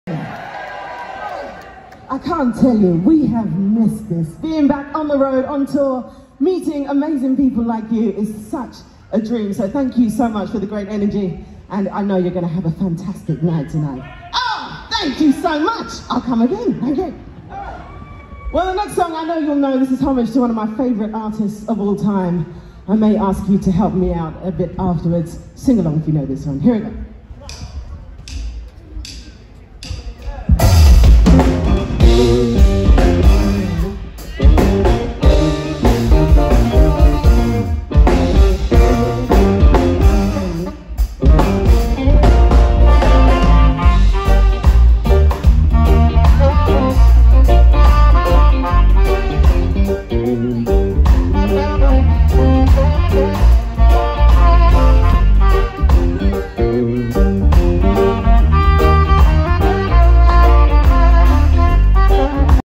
YolanDa Brown plays at Peterborough Embankment - We Love Peterborough
Playing live at The Embankment Peterborough Saturday 11th June 2022 it’s YolanDa Brown.
What magnificent music and a talented player. She's fabulous - never seen her before, but if see comes again I'm getting tickets again.